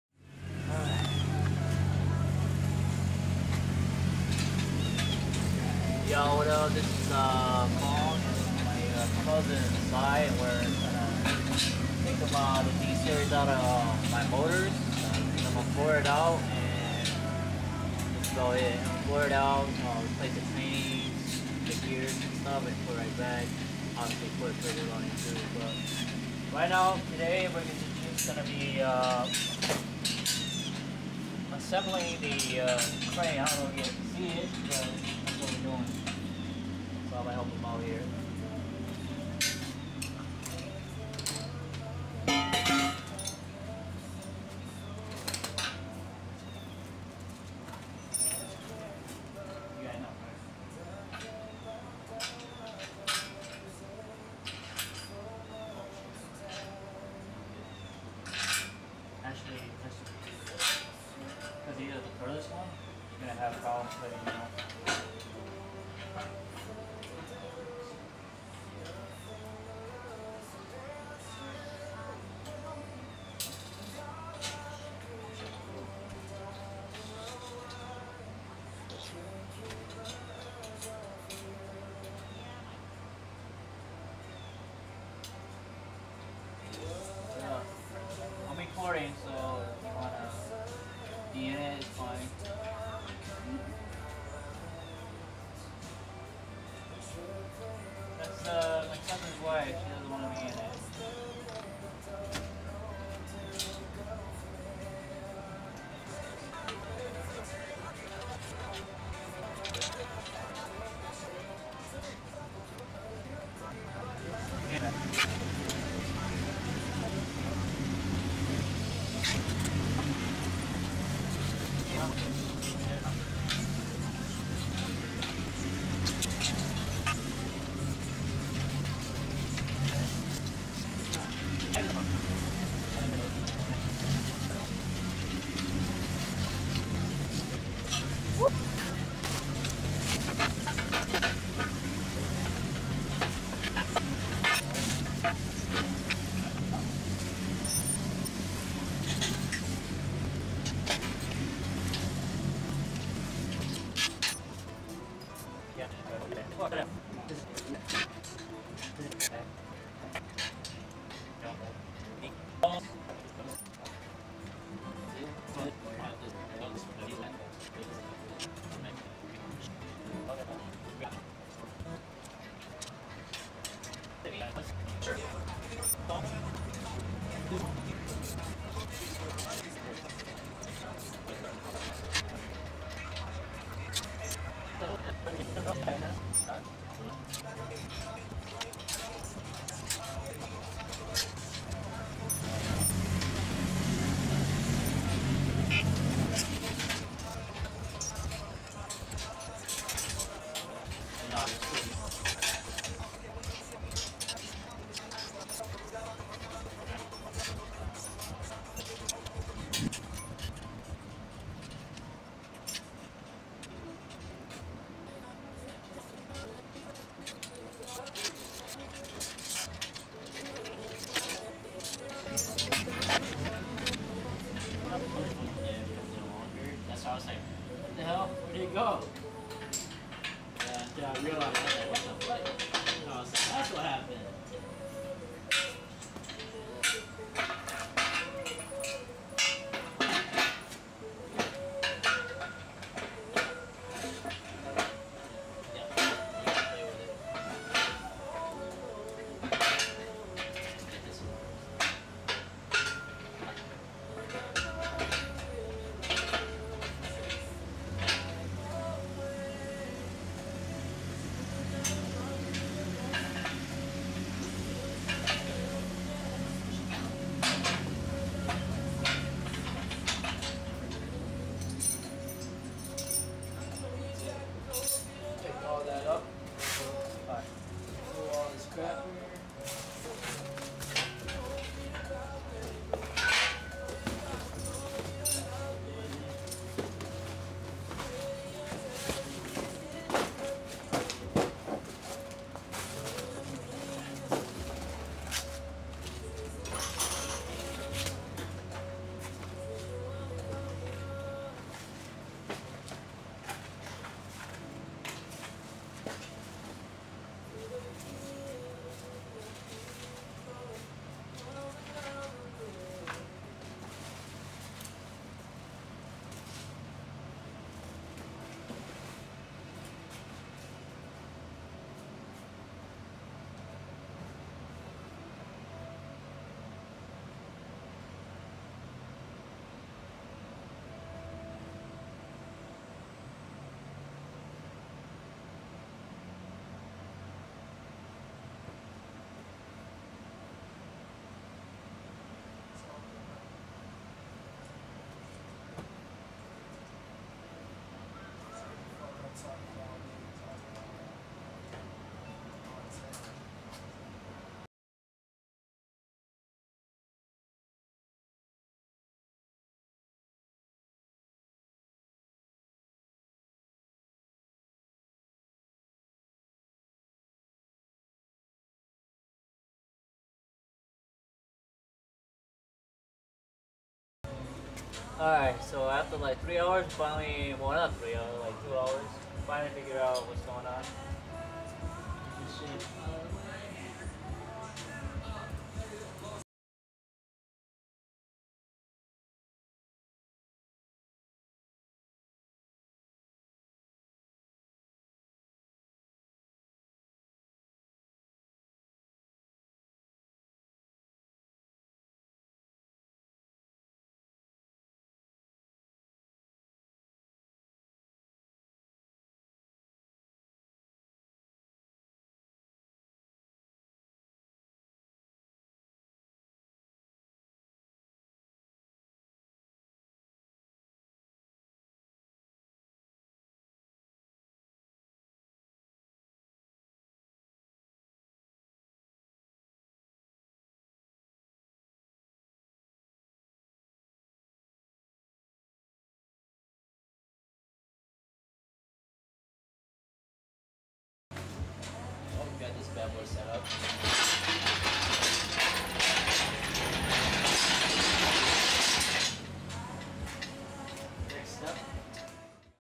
Engine Crane Assemble S1EP1
My name is Kong Xiong. I had recently purchased a car. I need to get my D-Series motor out of my car to the shop. My cousin and I had to assemble the engine crane.

Thank you so much for your time and effort to watching my videos.